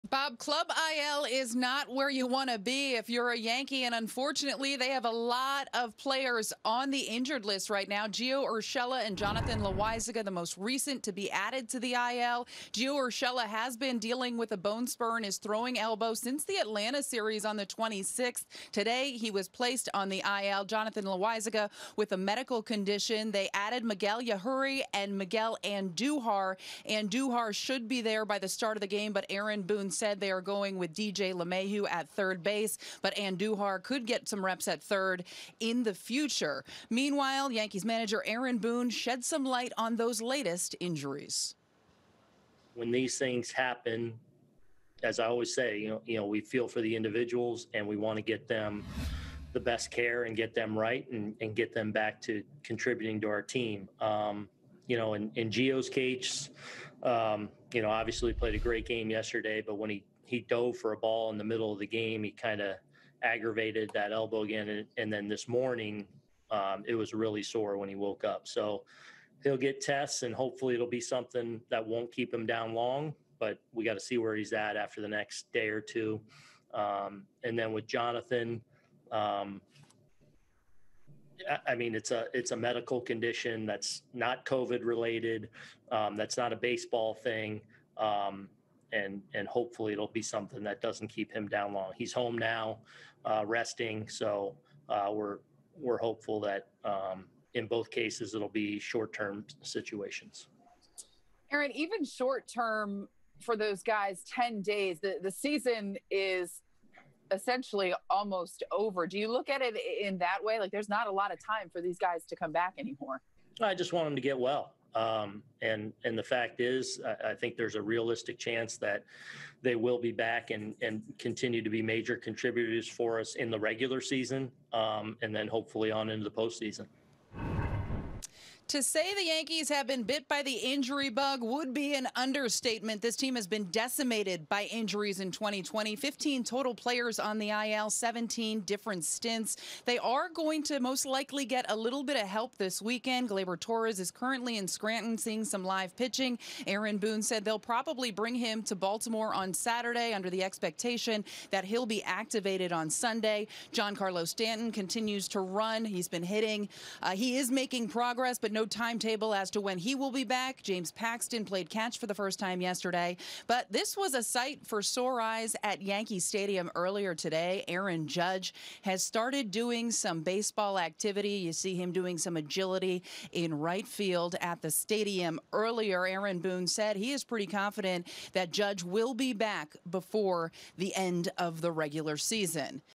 Urshela, Loaisiga head to I.L.; Judge continues progressing
Meredith Marakovits details the latest roster moves for the Yankees as two players head to the 10-day injured list, while Aaron Judge returns to the field in the Bronx on Friday.